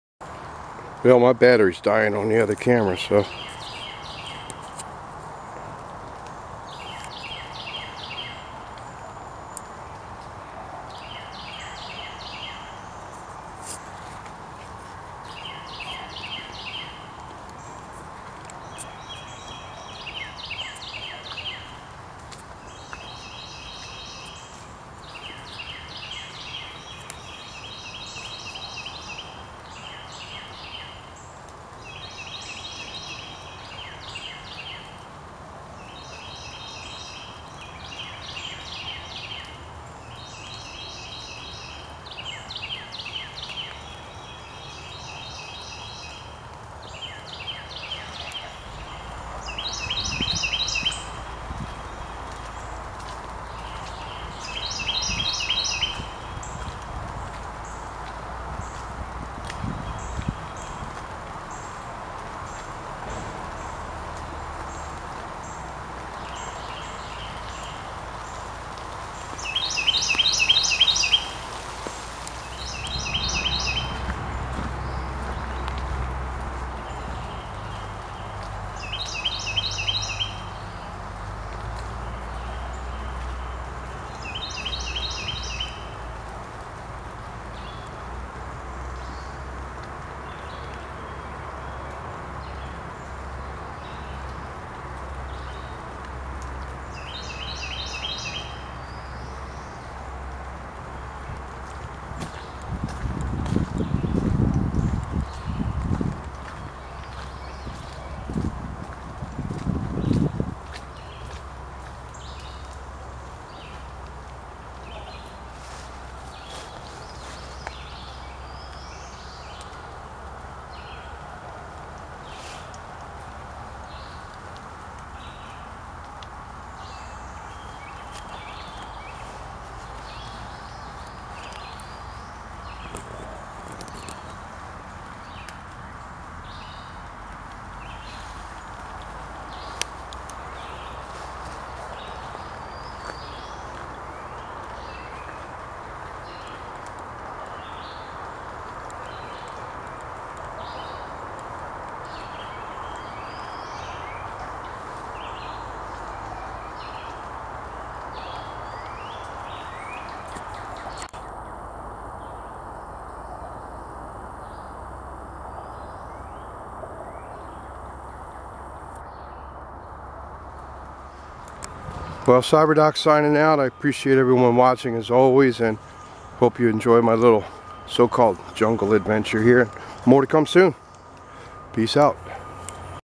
Inner City Jungle Adventure
A short walk through the woods early in the morning after a flood along the James River, Richmond, VA.